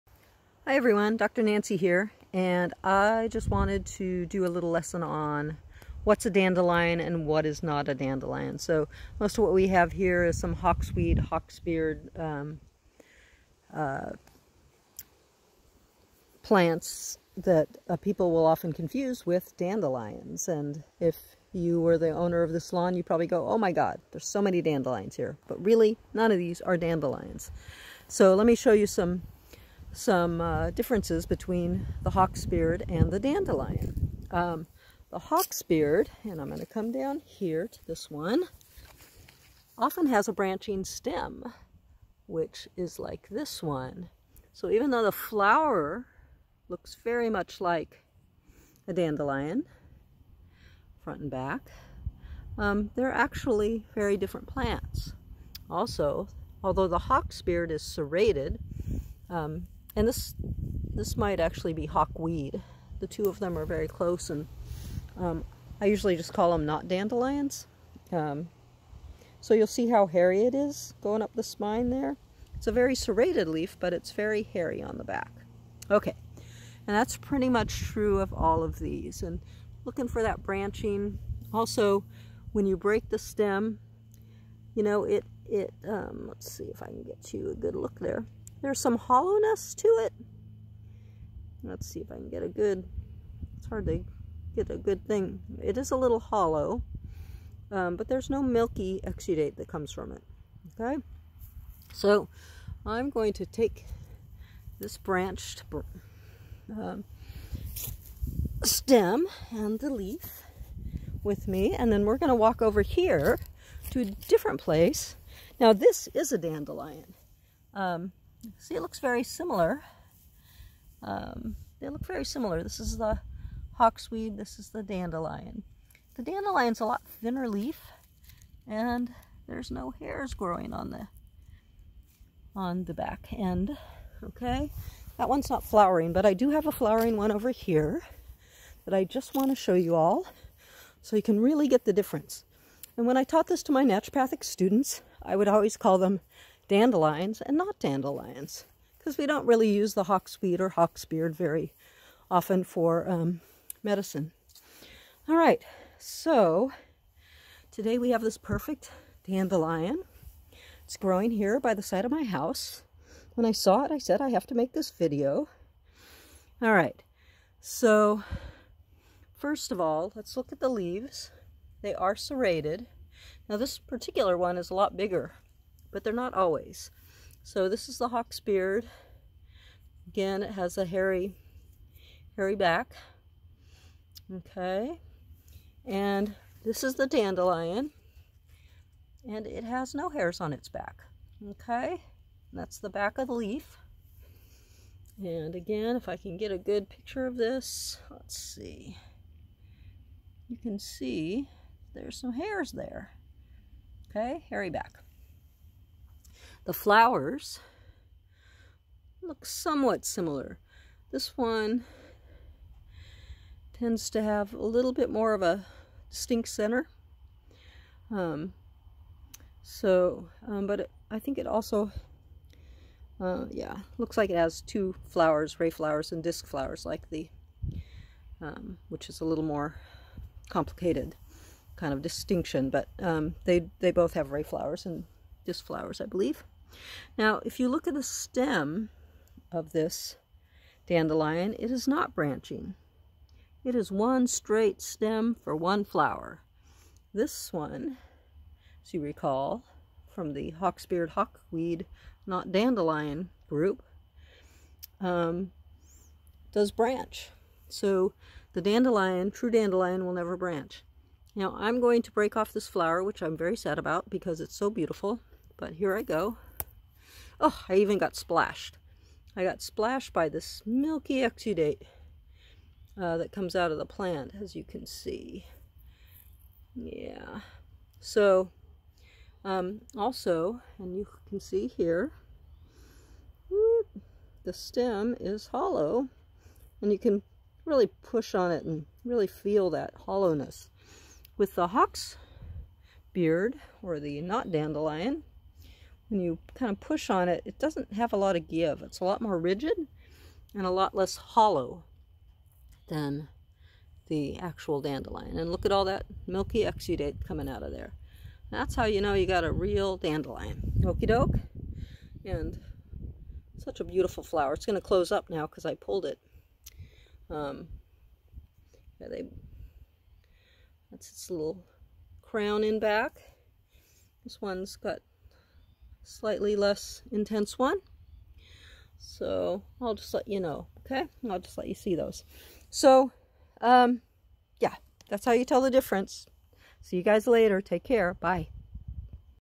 Dandelion and "not Dandelion". How to tell
Often we see beautiful yellow flowers and call the all "dandelion". They are often "not dandelions" but hawksweed or hawksbeard or a number of other similar appearing Asteraceae. Here's how to identify the dandelions.